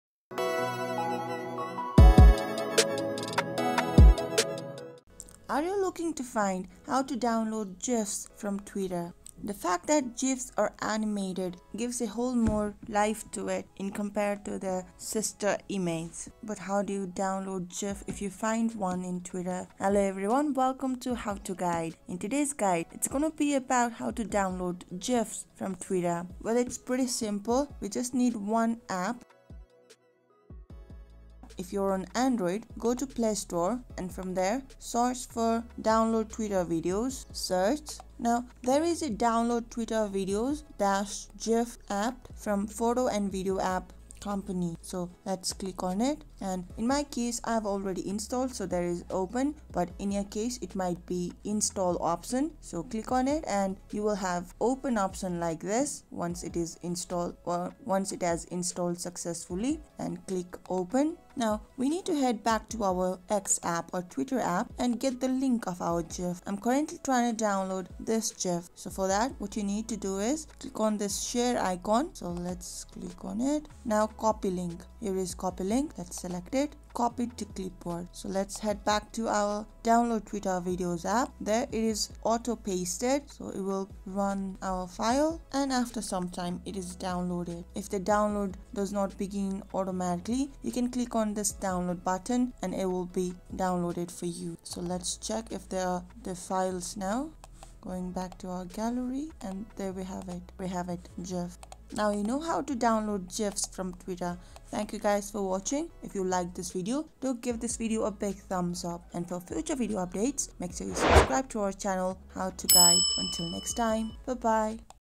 How to Download Gifs From X (2024) | Download Gif on Twitter?
Ready to up your GIF game? Dive into this video to discover the quick and easy way to download GIFs from Twitter!  Whether you're looking to add some flair to your messages or want to save that hilarious GIF for later, we've got you covered. Watch now and elevate your Twitter experience!